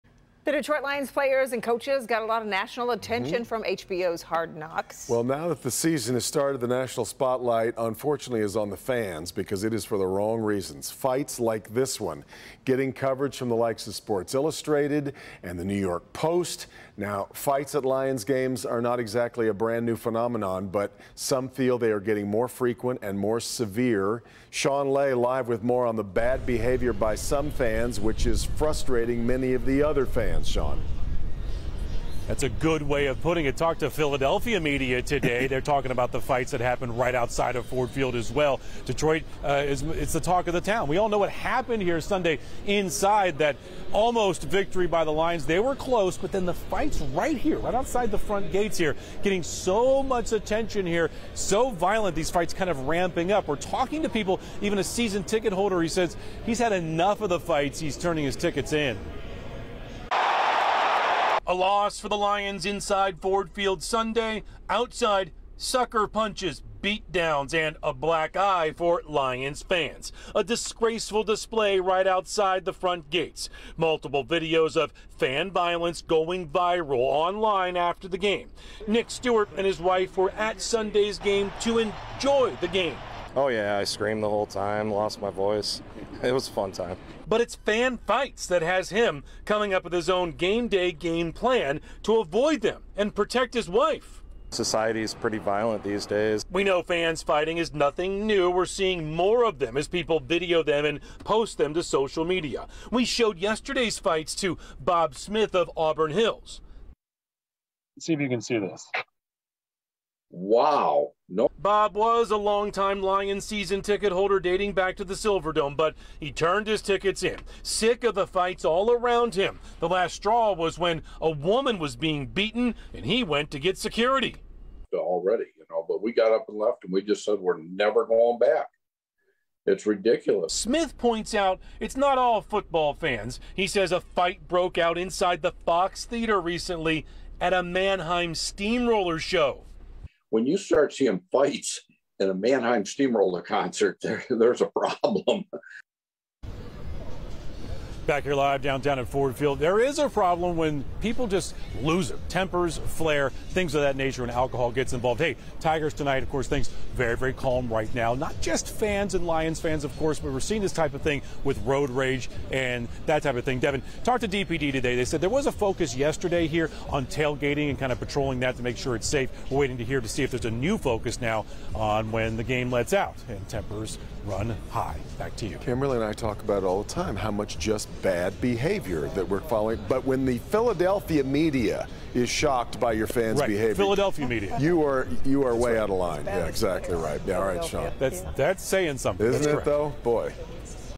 Bad behavior by Lions fans has growing frustration for some at Ford Field
The Detroit Lions players and coaches got much national attention from HBO's Hard Knocks.   Now that the season's started, the national spotlight is on the fans for the wrong reasons.  Fights like the one seen in the video player above are getting coverage from Sports Illustrated and The New York Post. The fights aren't a brand new phenomenon, but some feel they're getting more frequent and severe.